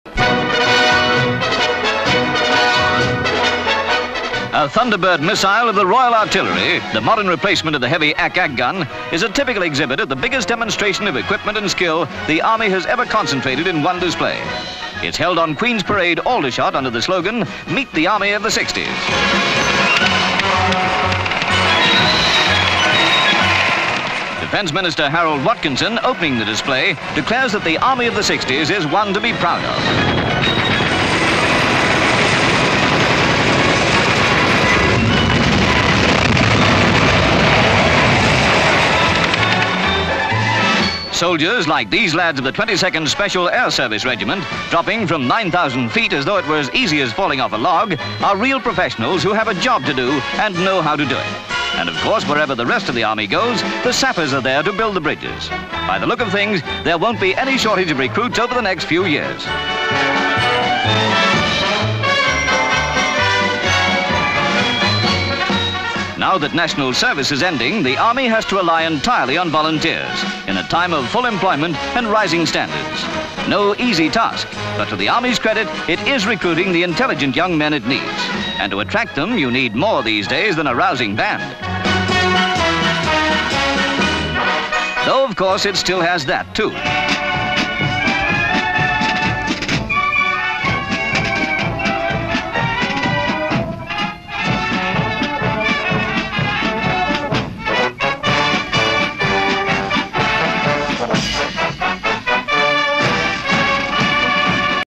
Army Of The Sixties (1962)
Army of the sixties. Aldershot in Hampshire.

L/S Thunderbird missile. L/S A group of missiles. L/S Missile being loaded onto transport. L/S of the complete show, with crowds looking round. L/S Crowds seated. L/S Two helicopters carrying troops. L/S Viscount Watkinson, (Defence Minister with Major General Metcalfe, G.O.C. L/S Troops shinning down ropes from the helicopters. L/S Troops running across the field through smoke screens. C/U Small boy watching. L/S As a mock radar jamming station is blown up.

M/S People looking up. L/S Paratroops of the 22nd S.A.S. Regiment coming down. M/S Man looking through long telescope. L/S As one of the parachutists lands. L/S Crowd.  M/S Two of the parachutists talking together. C/U One of them. L/S The army demonstrating a bridge laying machine. M/S Boys playing on a rope net. M/S Pan as mall boy swings on a rope and lets himself into a net. M/S Pan as another boy does the same. M/S Pan, boy coming down a parachute training slide.

L/S Crowds of children on a tank. M/S Pan, crowds of children on a tank. C/U Boy looking into the tank. M/S Army officers watching, centre one is Major General Metcalfe. L/S and M/S Another group of boys on a tank. L/S Crowd. L/S the massed bands and drums of the Aldershot District marching past. M/S Tracking shot, drummers leading the band. L/S Tracking shot, the massed bands.
 FILM ID:1721.11